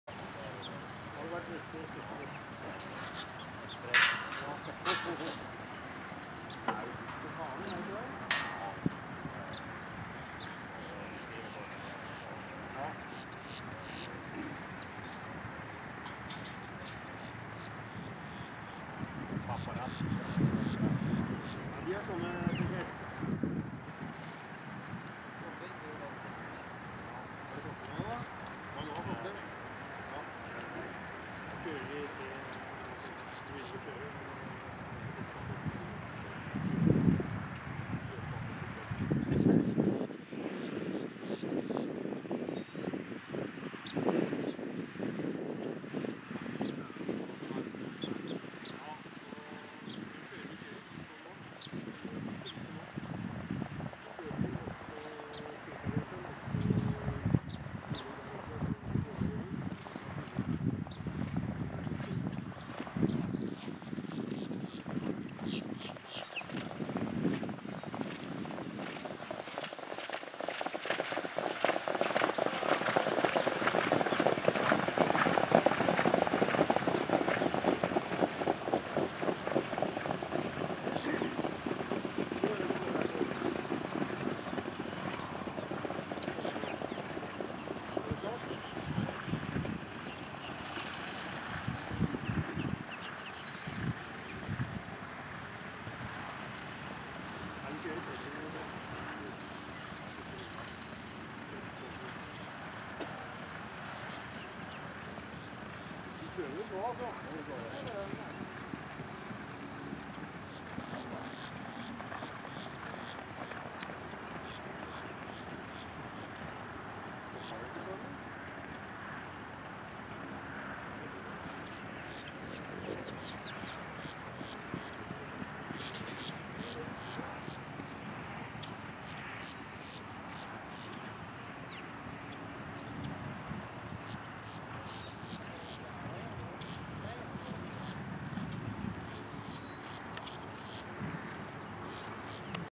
Svanstedt Stable Fri 6 Mars Wegottatruckyea,Bahar H,Cubit,Webify H,Centurion Atm,Ice O late
Here are we going one heat the wrong way.
In the Lead is pacer Wegottatruckyea(rocknroll Hanover-Magic of Michelle) outside of him is Bahar Hanover(Donato Hanover-Bone A Fide) in the pocket is Cubitt(Credit Winner-Dornello) second over is Webify Hanover(Muscle Massive-Whitesand samantha)
third inside is Cetrurion Atm(Sj's Caviar-Ens Tag Session)
and Third over is Ice O Late(Yankee Glide-The Ice Queen)
All is 2 years old training at Svanstedt Stable Vero Beach Florida